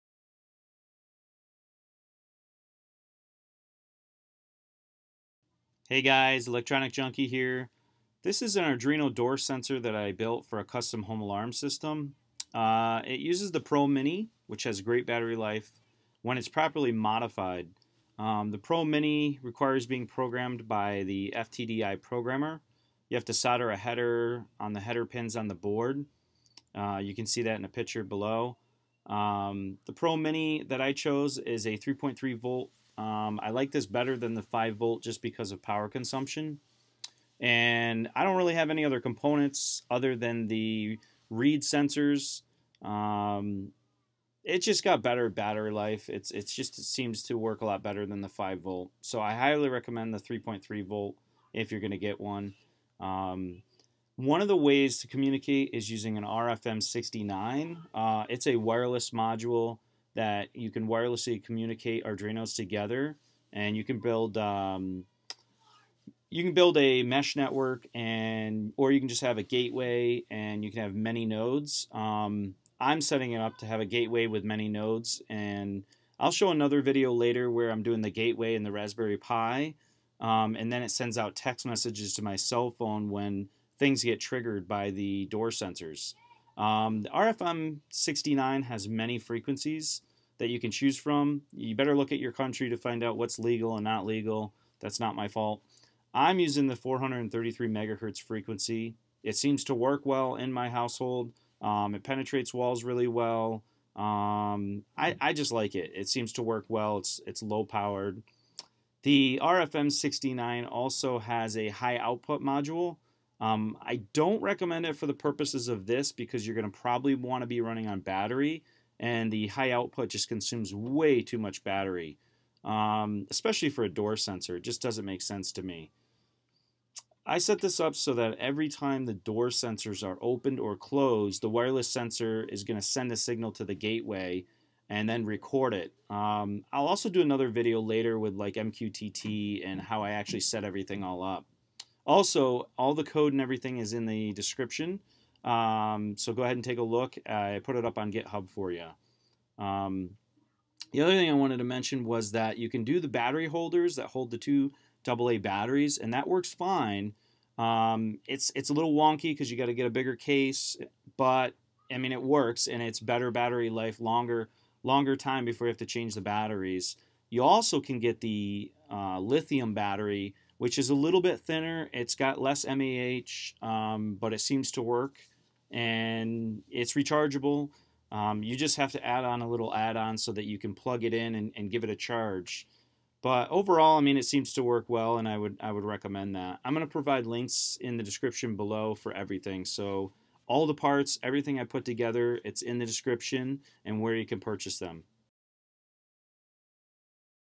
Arduino Door Sensor
Arduino pro mini 3v wireless door sensor for a custom home alarm system.
Arduino wireless door sensor
Arduino pro mini door sensor
Arduino home alarm sensor
Alarm door sensor custom

$3.90 - wireless module 433mhz

Qty 5  - $8.54 ($1.71 ish each) arduino pro mini

$2.99 - tinned wires for soldering

$2.40 - battery holder AA

$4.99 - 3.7V 1500mAh Lipo li-Polymer ion Battery

$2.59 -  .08v to 3.3v step up converter